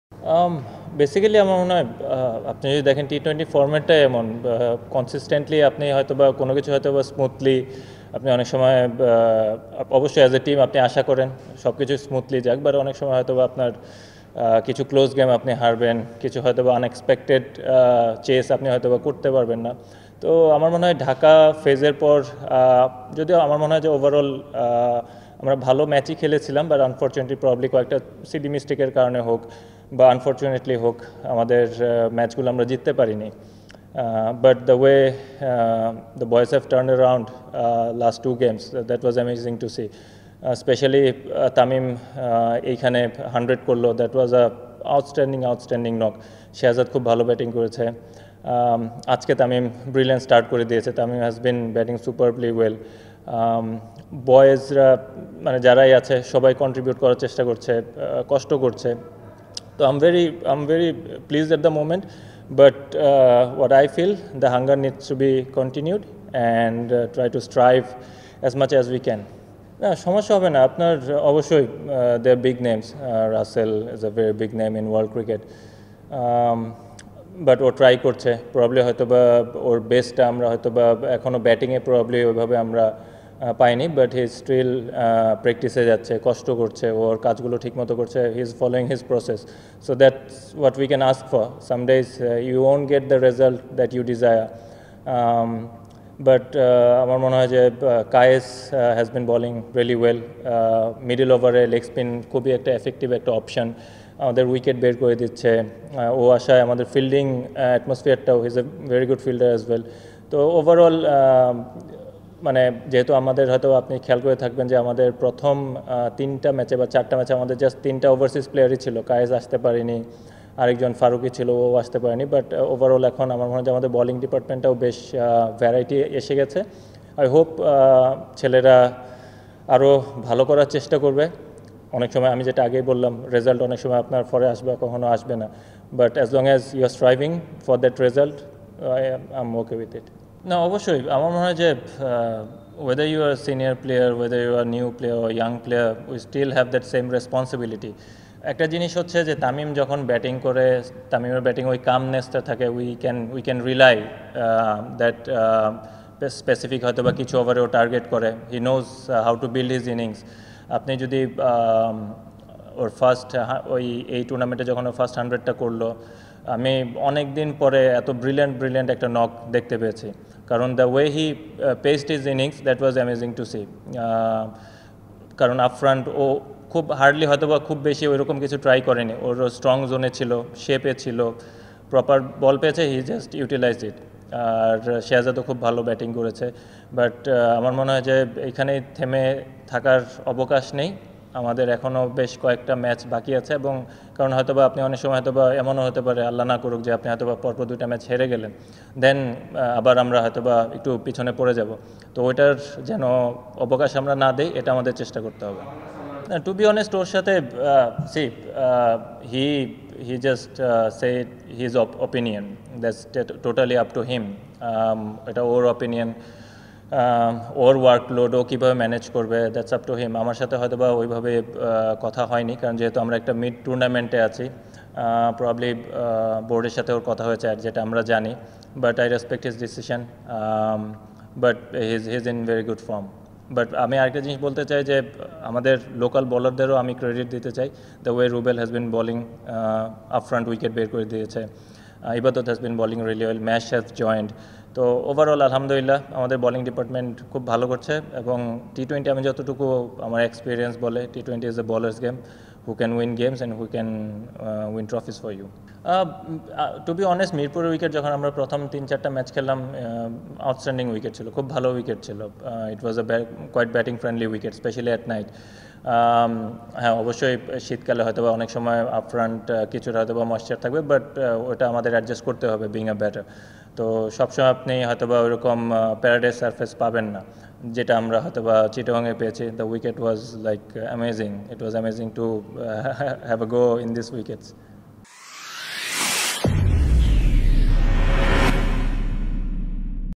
What did Mahmudullah say after recording the best batting of T20 life?
Minister Dhaka's captain Mahmudullah Riyadh set an excellent example of leading from the front. He played an unbeaten innings of 60 off 41 balls with the bat first. Later he also spent only 18 runs in 3 overs at a crucial time in bowling. On this day of all-round skill, Dhaka defeated table topper Comilla Victorians by 50 runs, Mahmudullah himself was selected as the best of the match.